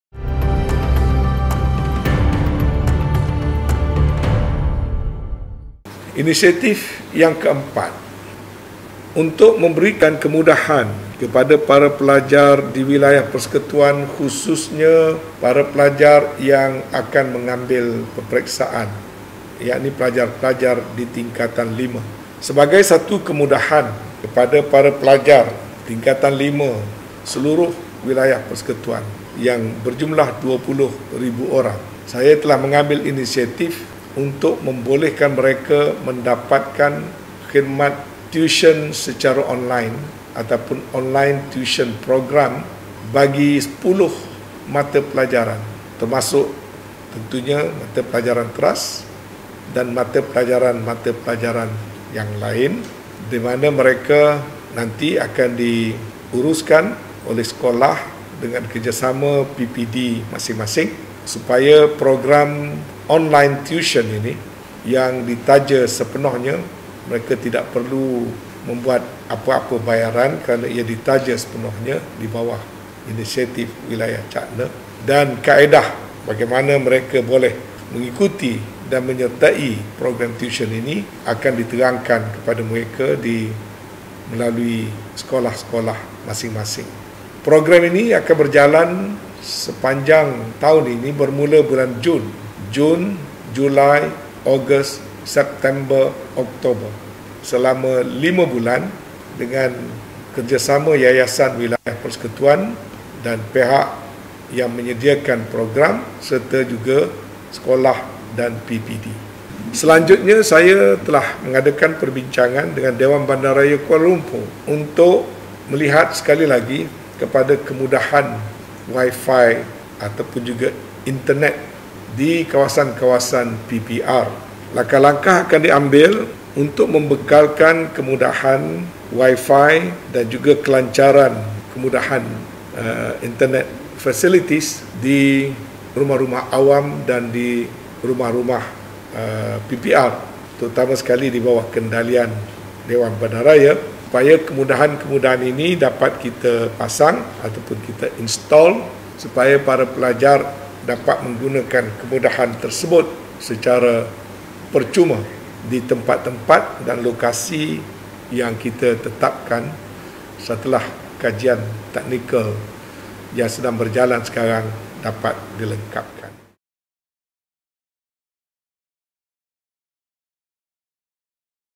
Tuisyen Percuma Kepada Pelajar Tingkatan Lima
Inisiatif Keempat Wilayah Cakna 5.0

Wartawan & Visual Editor: Hanim Nadia